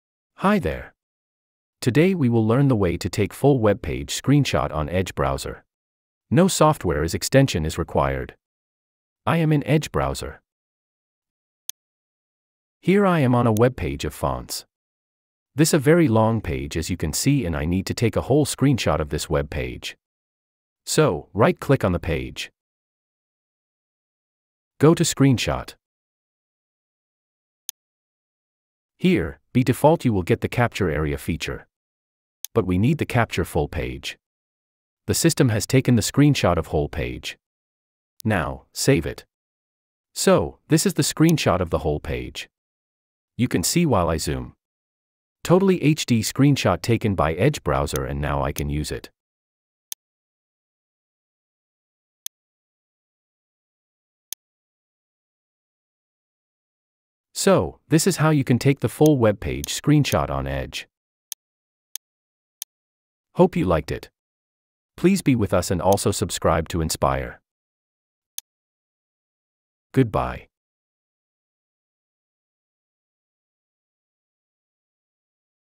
How to take Full Webpage Screenshot in Edge Browser - No Extension or Software
Tired of scrolling to capture an entire webpage?

Microsoft Edge has a built-in tool to capture FULL webpages, even the parts you can't see on your screen!  This video will show you exactly how to do it in just a few clicks.

Here's what you'll learn:
How to take a screenshot of an ENTIRE webpage in Microsoft Edge (easy!)
Using keyboard shortcuts for FASTER capturing (Ctrl+Shift+S)
Saving your screenshots to your desired location
This video is perfect for:

Students who need to capture online resources for research
Anyone who wants to save web articles for later reading (without ads!)
Professionals who want to share specific sections of a webpage

The video deals with the following ideas:
Microsoft Edge, Full Page Screenshot, Web Capture, Long Screenshot, Scrolling Screenshot, Capture Tool, How-To.

Microsoft Edge offers a convenient feature called Web Capture, which allows users to take full-page screenshots effortlessly. This tool is perfect for creating long or scrolling screenshots, enabling you to capture the entire content of a webpage in one go.

To use this capture tool, simply click on the three dots at the top right corner of the browser, select "Web Capture," and choose "Capture full page."
This how-to method ensures that every part of the webpage, from top to bottom, is included in the screenshot, making it ideal for preserving lengthy articles, tutorials, or any web content that extends beyond the initial screen view.